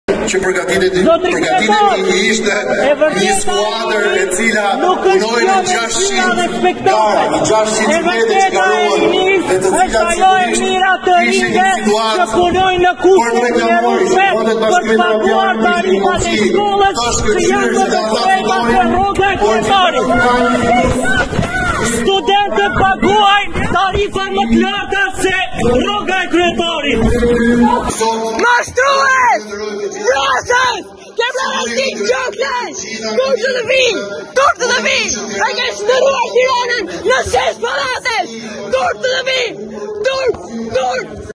“Betonizues i Tiranës e vrasës i Ardit Gjoklajt” studentët i prishin aktivitetin Veliajt
Video Credits: Lëvizja për Universitetin

Studentët i kanë prishur aktivitetin e organizuar ditën e sotme nga kryebashkiaku Erion Veliaj për Ditën e Rinisë.

Gjatë fjalës së Veliajt, studentët dhe aktivistët e Lëvizjes Për Universitetin janë ngritur duke thirrur nga salla se "Studentët paguajnë tarifa më të larta se rroga e kryetarit!".

Ata e kanë akuzuar edhe si betonizues të Tiranës dhe vrasës të Ardit Gjoklajt, para se të ndërhynin rojet e sigurisë dhe t’i shoqëronin jashtë.

"Studentët paguajnë tarifa më të larta se rroga e kryetarit!" Kjo ishte thirrja që bënë aktivistë të Lëvizjes Për Universitetin sot, në aksionin e zhvilluar gjatë takimit propagandistik të Erion Veliajt për Ditën e Rinisë. E vërteta e rinisë nuk është cirku pa bukë që na ofrojnë në raste ''festash'', por tarifat e larta, cilësia e ulët e arsimimit, kostot e papërballueshme të jetesës dhe mungesa e shpresës! Erion Veliaj nuk është përgjegjës vetëm për mashtrimet ndaj studentëve, por edhe fajtor për vrasjen e 17-vjeçarit Ardit Gjoklaj!” – shkruan LPU.